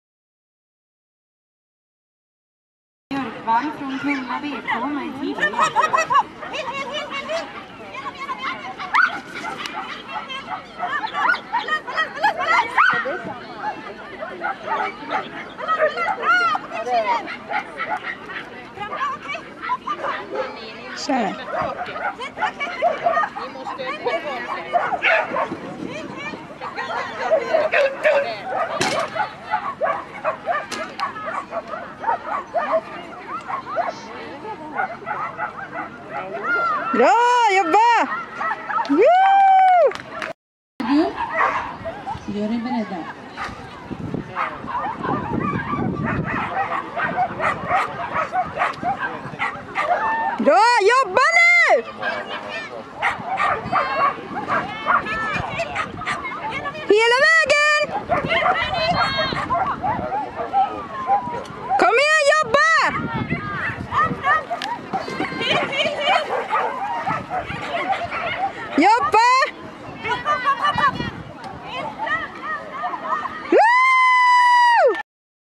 Hilja, Frövi 16 augusti 2014
Duktig tjej, två korrekta slalom och en pinne som bonus!